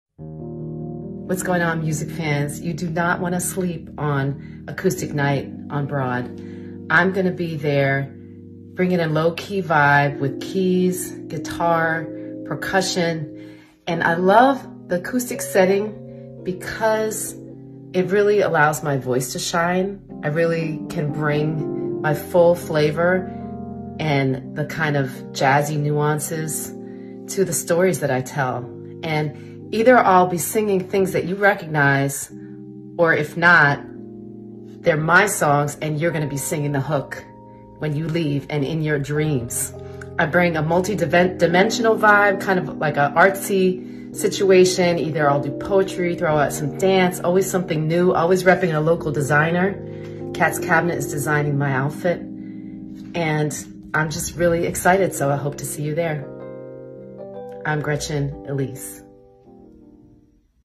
Acoustic Night on Broad 3-25-23 Gretchen Elise Promo Clip
Acoustic Night on Broad is back again! Did you get your tickets yet?

@gretchenelisemusic * @whoisnicobryant @othneilchamberssr @zavetis & @boogiemandela will be in the building performing live and unplugged!

*Gretchen Elise performs with Zavetis (guit), Jeffrey Hughes (keys) and Jan Jeffries (percussion).

Hosted by @twanny2times
Produced by @stereotypesoundsessions and @Codegreenmedia

This is an exquisite performance where you will get an up close and personal look at every performer. Don’t miss this!  Be prepared for a night of beautiful music, great food and an overall awesome time in an intimate setting.

Saturday
March 25
6pm - 9pm